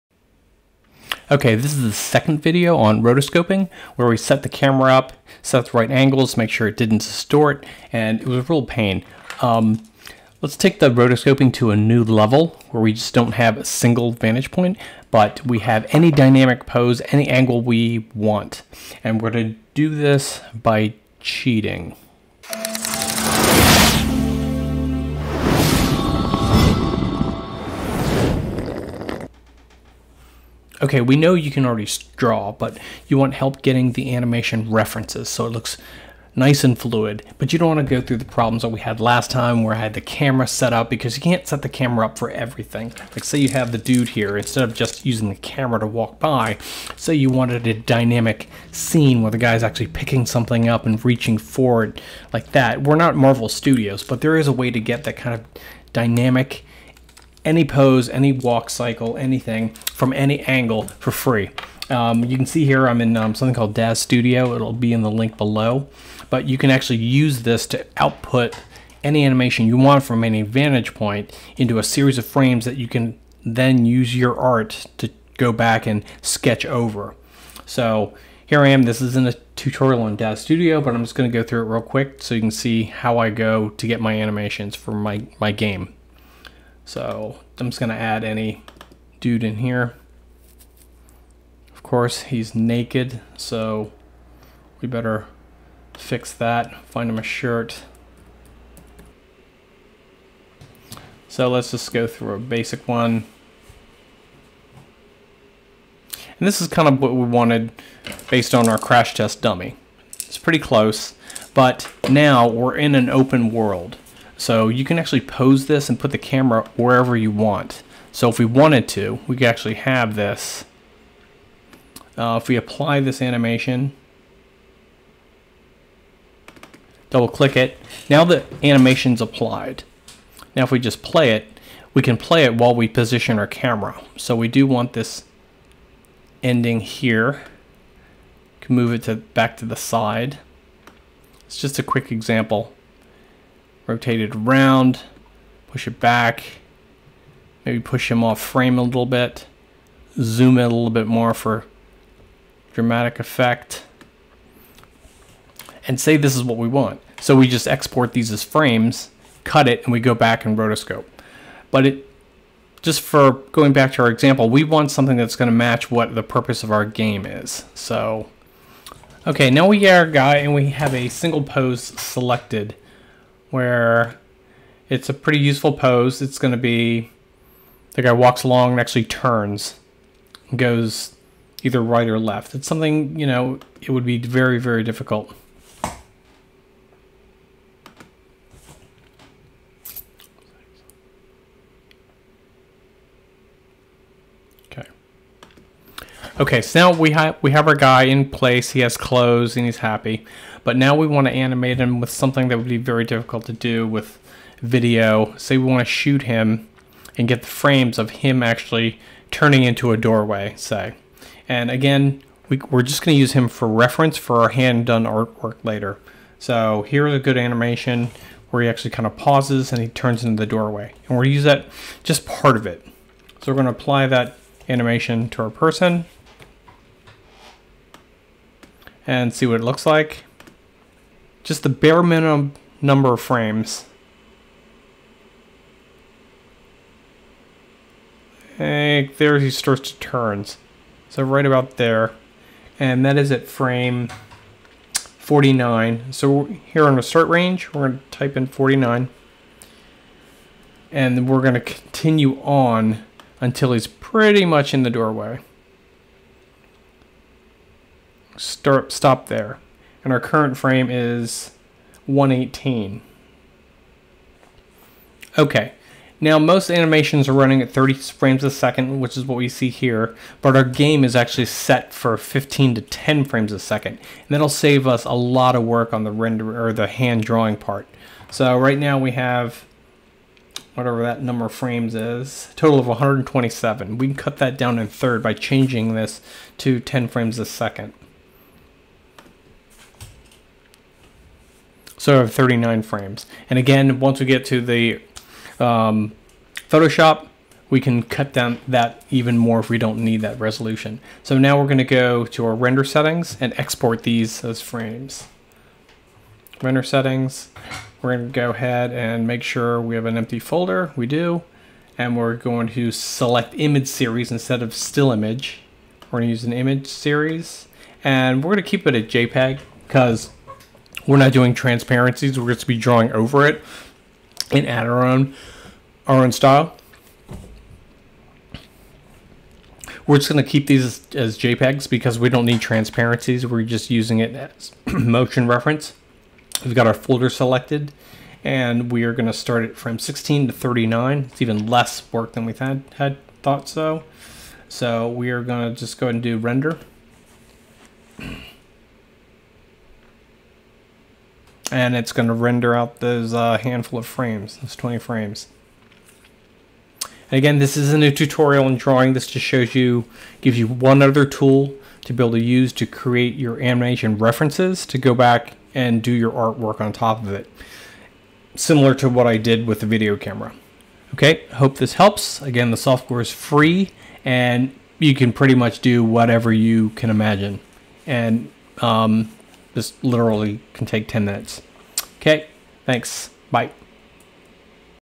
Rotoscope Animation Tutorial - Next Level with 3D Software!
My 2nd video tutorial on rotoscope animation - get to the next level with free 3d animation software.

The animation software does the dirty work of camera placement and premade motion capture libraies, you just set the scene and render out the reference frames. What you do with the virtual camera placement and post-production sketching is only limited by your imagination! Let do this!